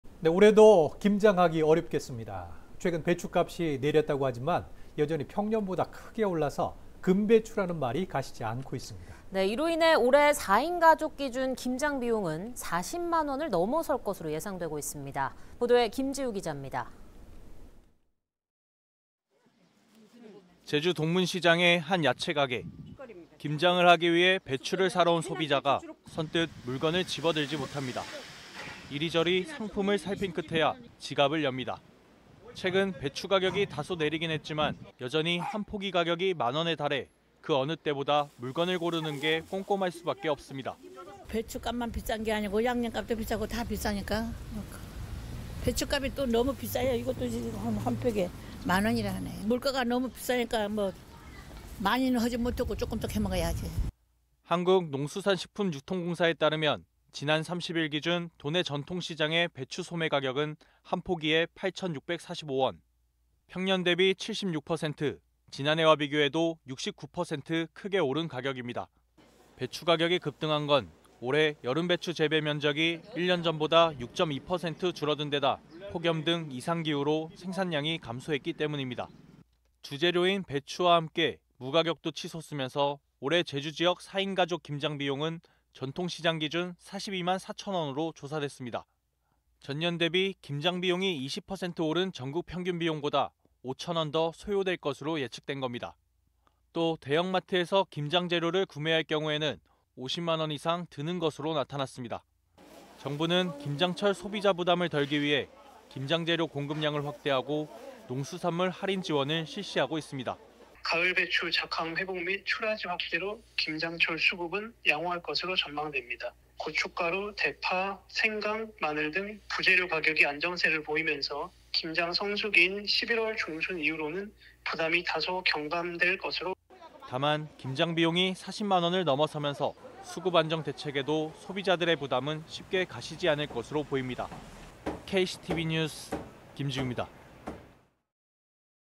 내려도 '금배추'…김장비용 40만원 '훌쩍'
최근 배추가격이 다소 내렸지만 여전히 평년보다 크게 높아 금배추라는 말이 가시지 않고 있습니다.

이로 인해 올해 4인 가족 기준 김장비용은 40만원을 넘어설 것으로 예상되고 있습니다.

제주동문시장의 한 야채 가게.

김장을 하기 위해 배추를 사러 온 소비자가 선뜻 물건을 집어들지 못합니다.

이리저리 상품을 살핀 끝에야 지갑을 엽니다.

최근 배추 가격이 다소 내리긴 했지만 여전히 1포기 가격이 1만원에 달해 그 어느 때보다 물건을 고르는 게 꼼꼼할 수밖에 없습니다.

[오정자 / 제주시 일도동]
"배춧값만 비싼 게 아니고 양념값도 비싸고 다 비싸니깐 배춧값이 또 너무 비싸요. 이것도 한 포기에 만 원이라 하네. 물가가 너무 비싸니깐 많이 하지는 못하고 조금씩 해 먹어야지."

한국농수산식품유통공사에 따르면 지난 30일 기준 도내 전통시장의 배추 소매가격은 1포기에 8천645원.

평년 대비 76%, 지난해와 비교해도 69% 크게 오른 가격입니다.

배추 가격이 급등한 건 올해 여름배추 재배면적이 1년 전보다 6.2% 줄어든데다 폭염 등 이상기후로 생산량이 감소했기 때문입니다.

주재료인 배추와 함께 무 가격도 치솟으면서 올해 제주지역 4인 가족 김장비용은 전통시장 기준 42만4천원으로 조사됐습니다.

전년 대비 김장비용이 20% 오른 전국 평균 비용보다 5천원 더 소요될 것으로 예측된 겁니다.

또 대형마트에서 김장재료를 구매할 경우에는 50만원 이상 드는 것으로 나타났습니다.

정부는 김장철 소비자 부담을 덜기 위해 김장 재료 공급량을 확대하고 농수산물 할인 지원을 실시하고 있습니다.

[임상민 / 한국물가협회 생활물가팀장]
"가을배추 작황 회복 및 출하지 확대로 김장철 수급은 양호할 것으로 전망됩니다. 고춧가루, 대파, 생강, 마늘 등 부재료 가격이 안정세를 보이면서 김장 성수기인 11월 중순 이후로는 부담이 다소 경감될 것으로…"

다만 김장비용이 40만원을 넘어서면서 수급 안정 대책에도 소비자들의 부담은 쉽게 가시지 않을 것으로 보입니다.